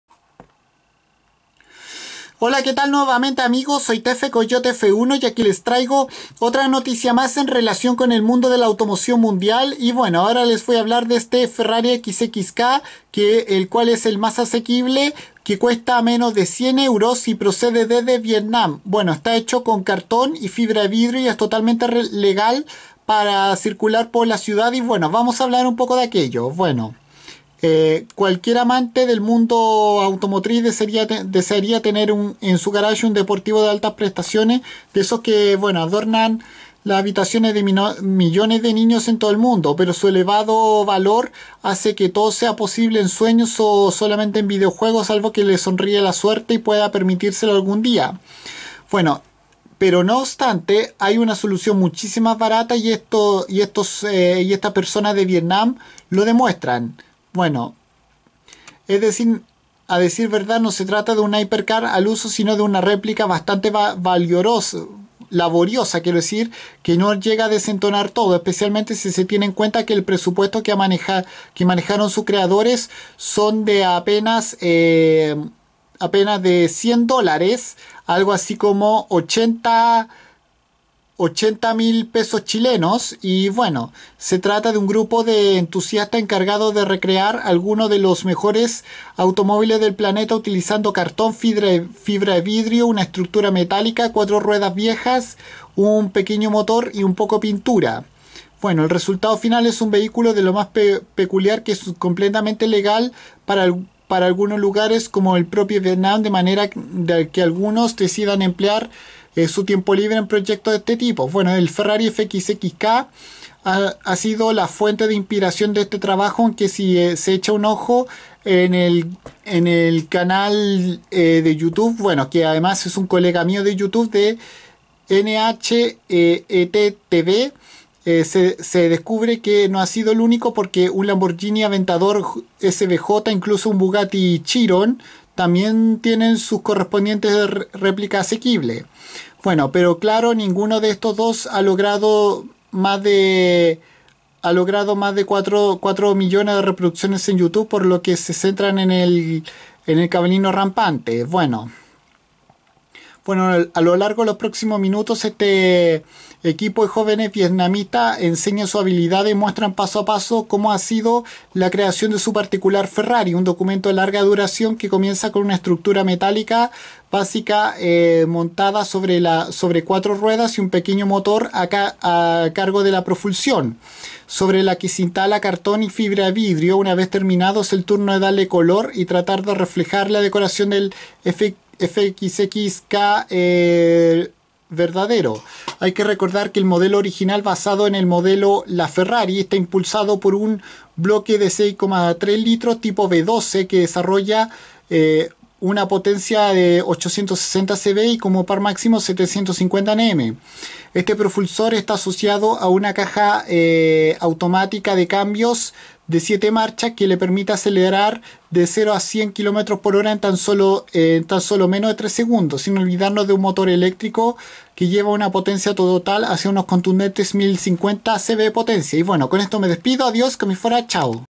Este es el Ferrari FXXK mas asequible y viene desde Vietnam
Esta recreación del exclusivo superdeportivo italiano de Maranello fue hecha de cartón y fibra de vidrio y les costó a sus creadores un grupo de jóvenes oriundos de Vietnam menos que el modelo real